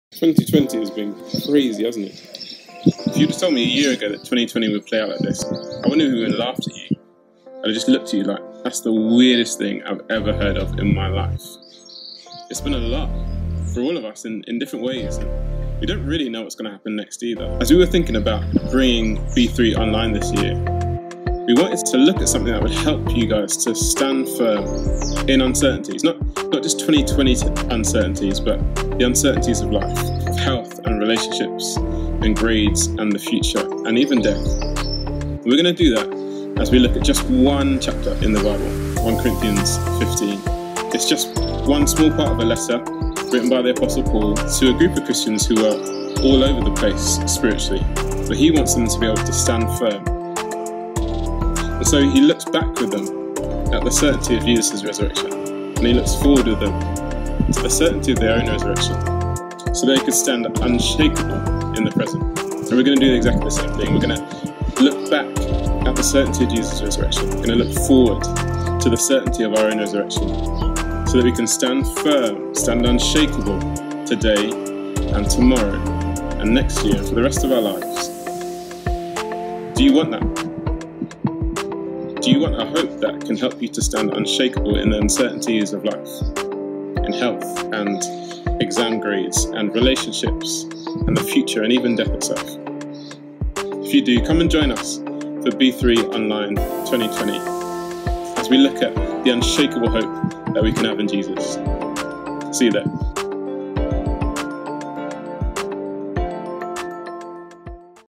Unshakeable 2020 Trailer
2020 has been a crazy year. This year, as B3 2020 goes online, we're going to hear how Jesus can help us stand firm.

Come and join the fun of B3 2020 Online!
Sign up with Ventures: brymorethree.co.uk/signup
Then create an account at brymorethree.co.uk